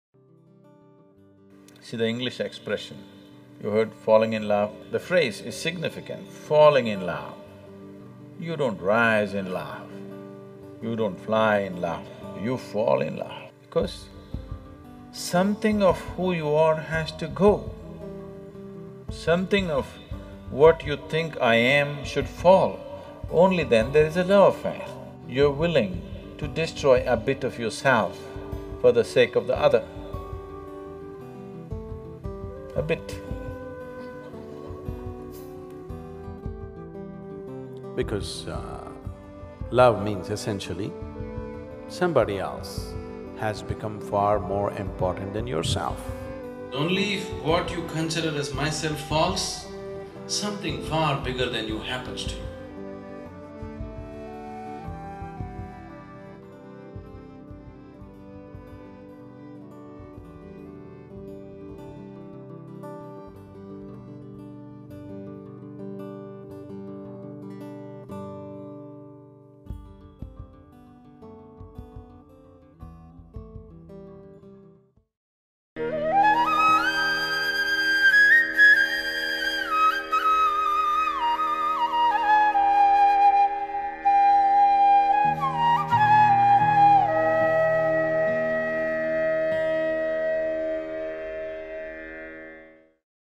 你是否曾坠入爱河？| Sadhguru 萨古鲁
萨古鲁解释了为什么“坠入爱里”这个词非常贴切，以及爱意味着什么。